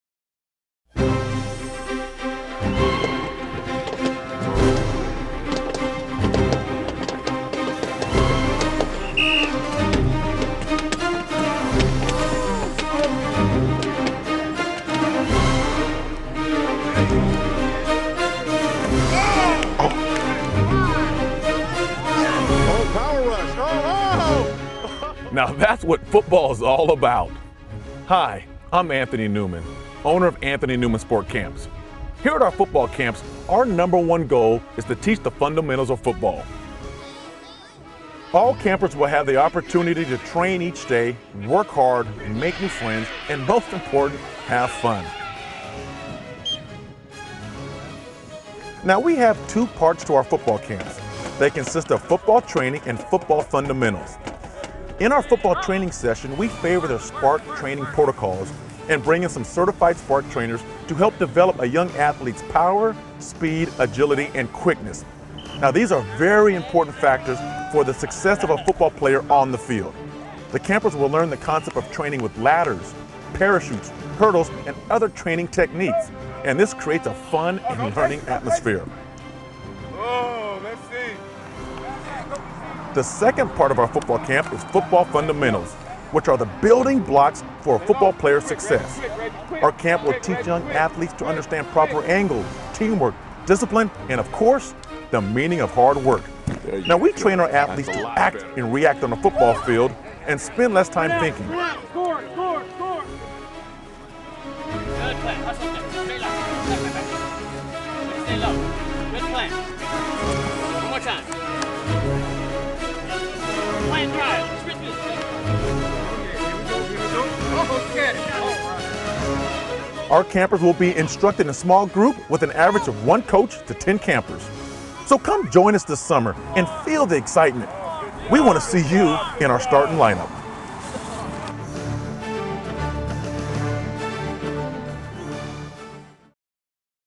Anthony Newman's Football Camps
Sport camps provide children a chance to develop good character, learn valuable life skills, discover new interests and make friends. By using all of our available resources, ANSC will provide a fun environment that offers safe, high quality activities supported by a trained and friendly staff.